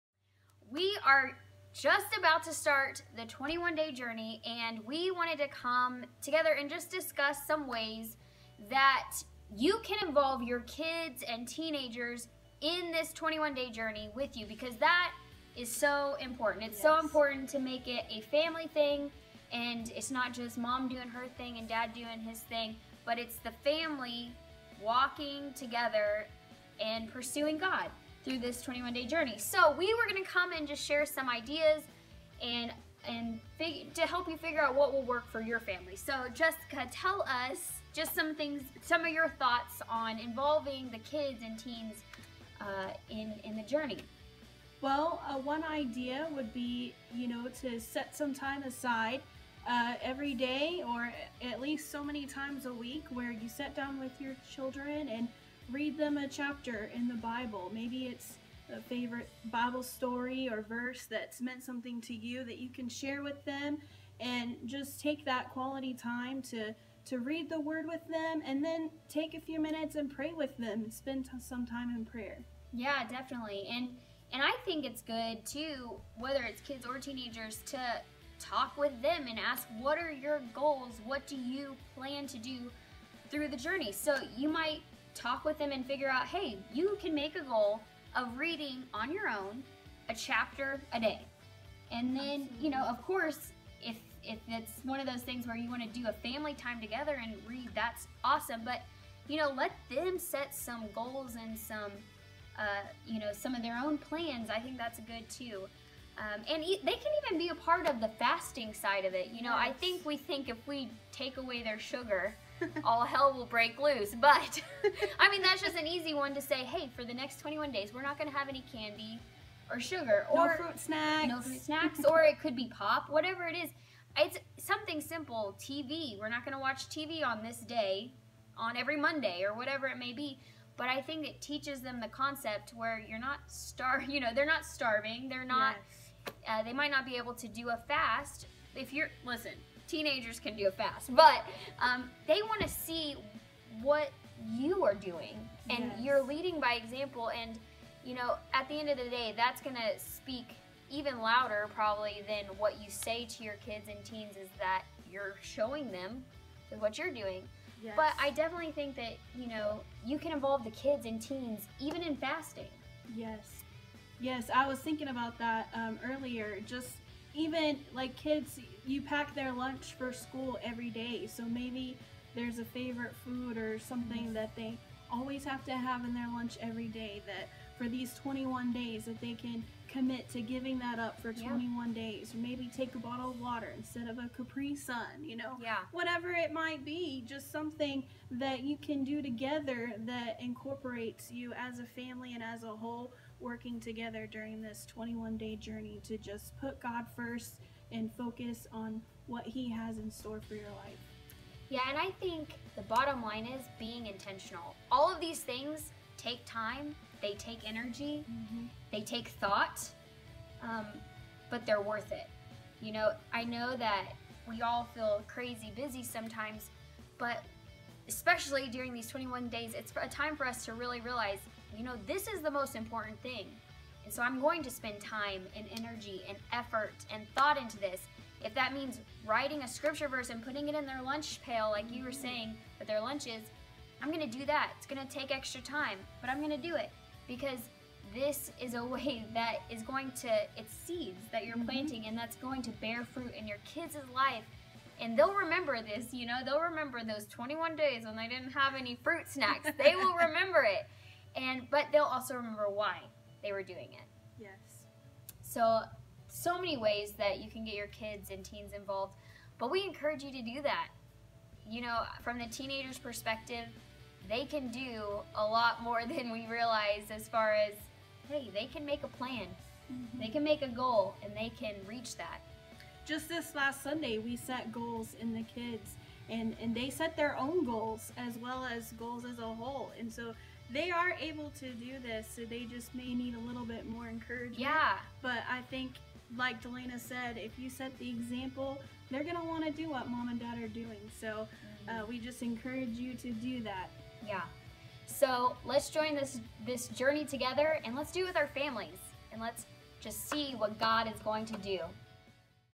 21 Day Journey: Better Together
In this video Pastor Dalayna and Pastor Jessica discuss what it looks like to get the whole family involved in the 21 Day Journey. Whether you have kids or teens there are some great and powerful ways to get them involved in these 21 days of devotion and consecration.